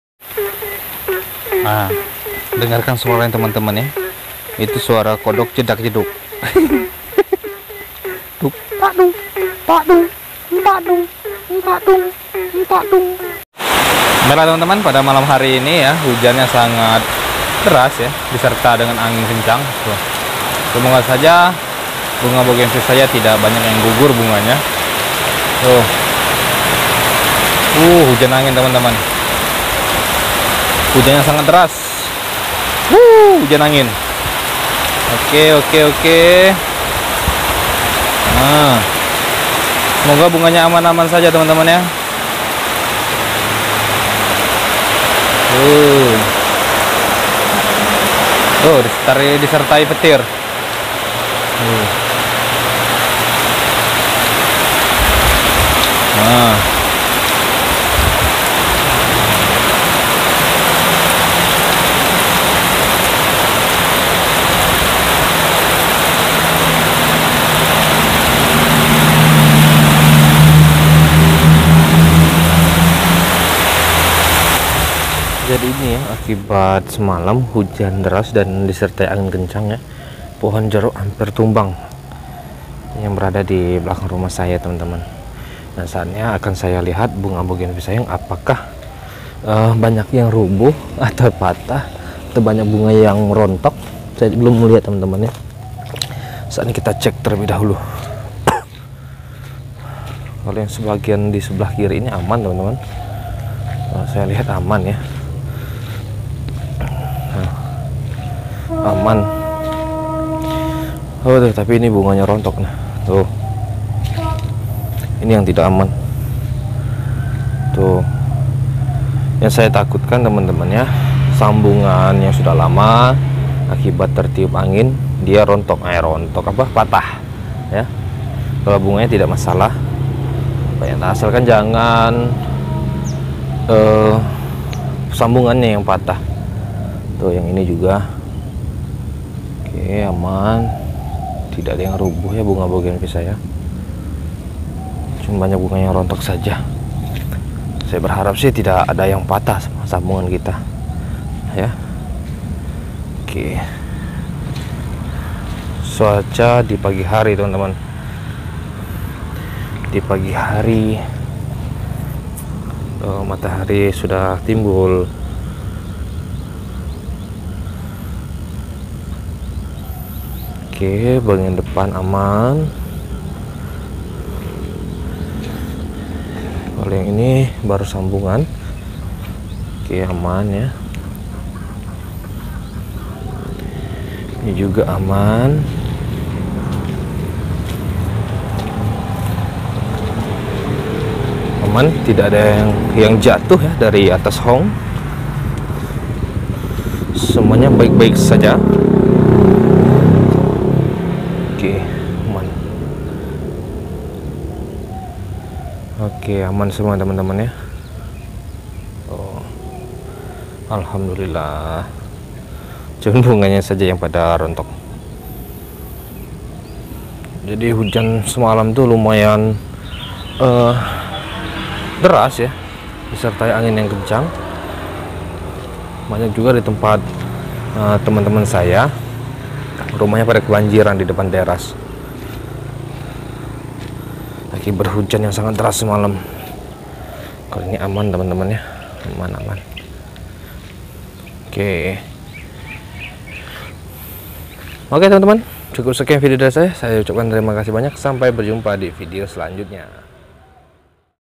BUNGA BOUGENVILLE DI GUYUR HUJAN DERAS || CUKUP SAYA SAJA YANG MERASAKAN
Hallo taman-teman Bougenville dimanapun kalian berada salam satu hobi.

Tepatnya hari Selasa 5 April 2022 malam Rabu di tempat saya diguyur hujan yang sangat deras disertai angin yang sangat kencang dan menerpa Bougenville saya, dan alhamdulilah Bougenville saya baik baik saja,cuma bunga nya saja yang rontok tapi itu tidak masalah nanti juga akan berbunga kembali.